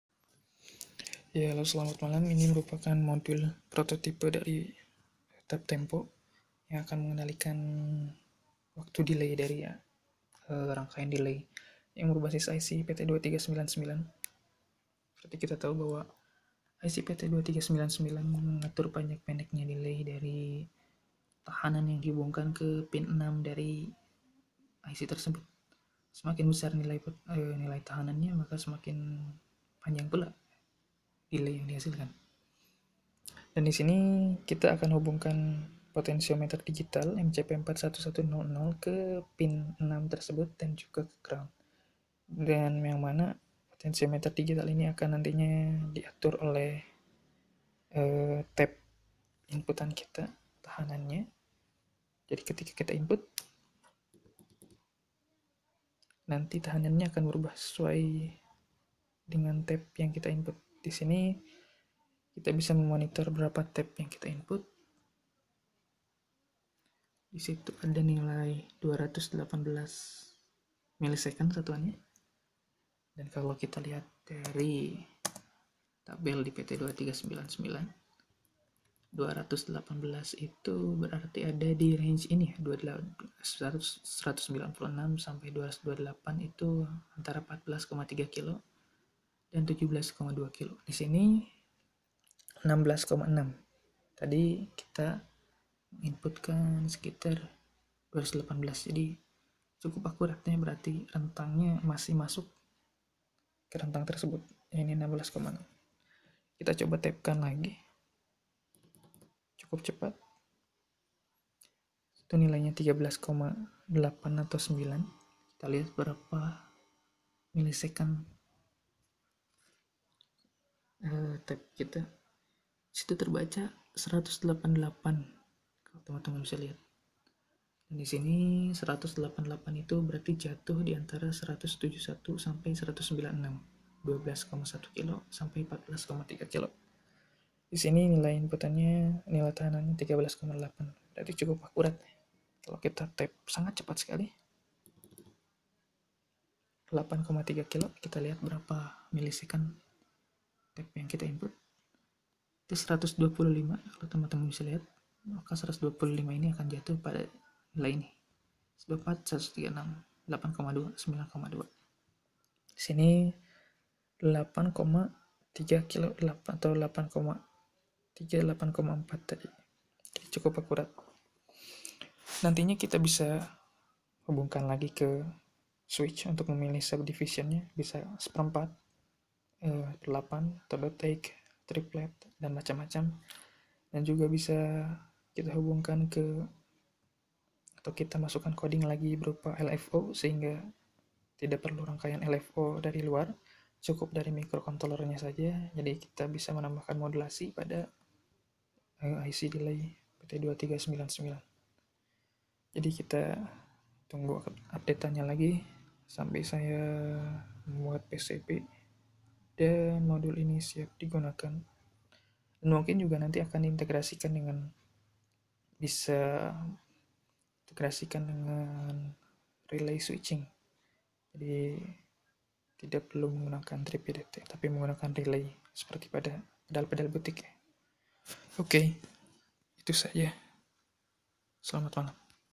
tap tempo module - prototype
This is my recent project, an attempt to create tap tempo module for PT2399 based delay.
The mcu store the interval tap and converted it to the corresponeding resistance based on the reference table in datasheet via digital potentiometer. Next, i'll add selectable subdivision feature.